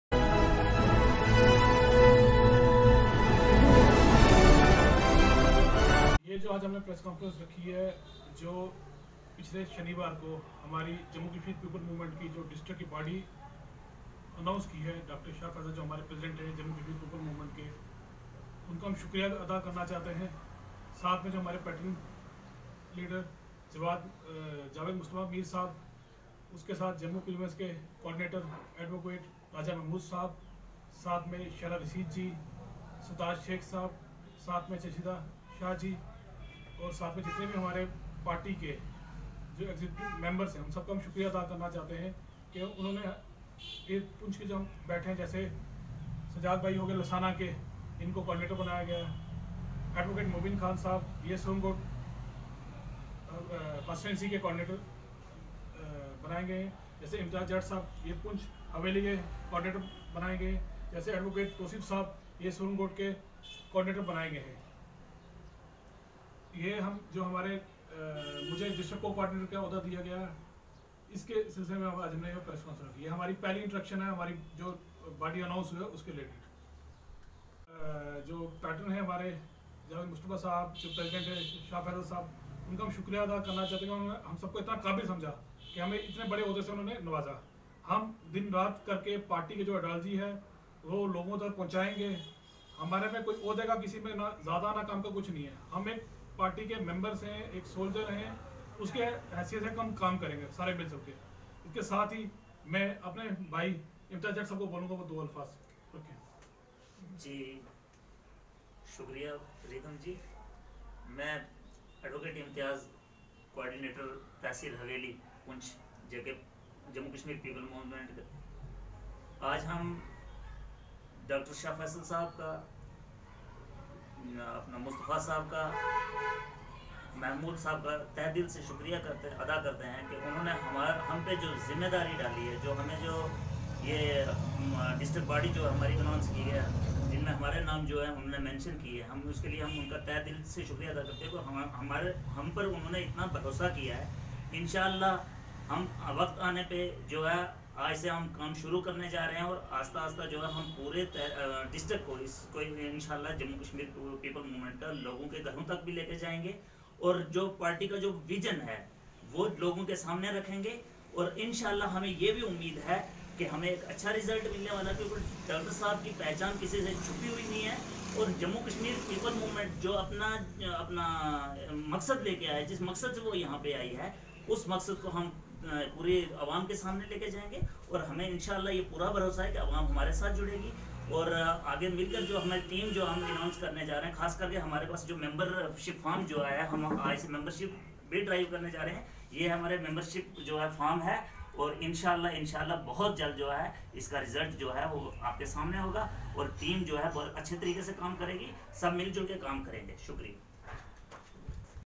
JK First News, Today JKPM Poonch held first meeting of Core body under Rydhampreet Singh Poonch T
Today JKPM Poonch held  first meeting of Core body under Rydhampreet Singh
Poonch
Today the core members of the JKPM Poonch in it's maiden meeting thanked Party president Dr Shah Faesal sahb, Javed Mustafa Mir sahb, Adv Mehmood Khan sahb, Sajad sheikh sabh, Chasfeeda Shah ji for reposing faith in them and giving them this important responsibility. They assured that they will continue to work as the foot soldiers to strengthen and spread the vision of Dr Shah Faesal. This meeting was held under presidentship of Rydham preet the District Co-Cordinator Poonch as Dr Jameel District Coordinator could not attend the meeting due to some personal reasons. Rydham preet Singh assured that in the coming days a full fledged membership drive will be organized in District Poonch. Others who spoke were  Adv Mobin khan Coordinator Surankote constituency, Adv Touseef Akram convenor Surankote Tehsil, Adv Imtiaz Jatt Coordinator poonch Haveli and others.